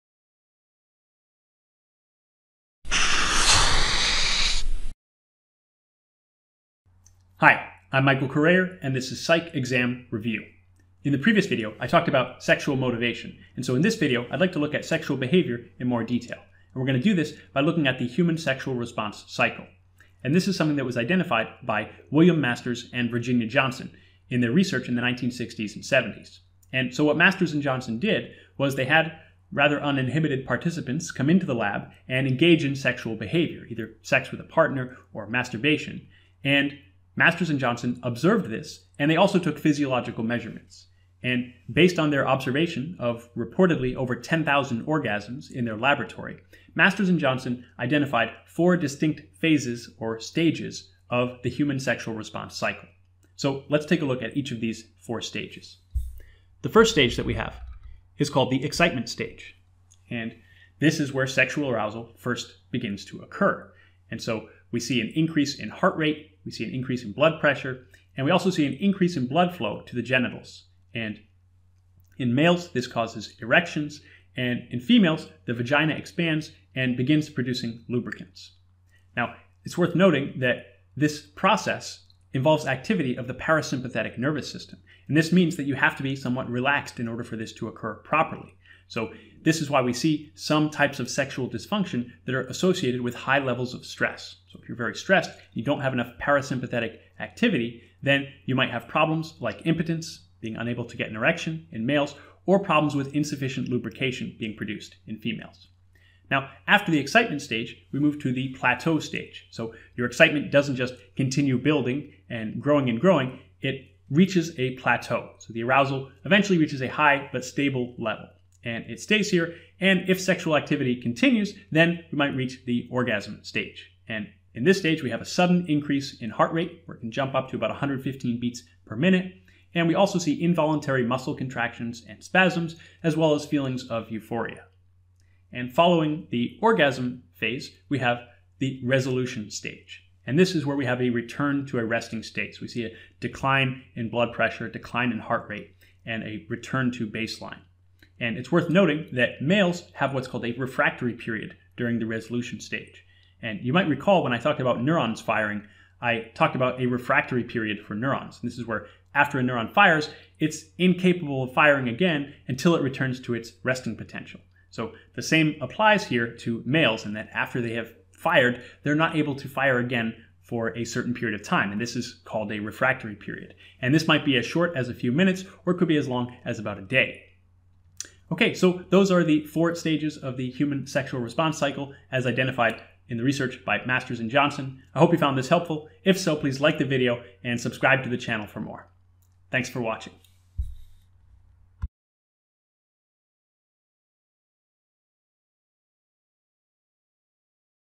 Masters & Johnson's Human Sexual Response Cycle (Intro Psych Tutorial #165)
In this video I briefly describe the work of William Masters and Virginia Johnson in identifying 4 distinct phases of the human sexual response cycle: excitement, plateau, orgasm, and resolution.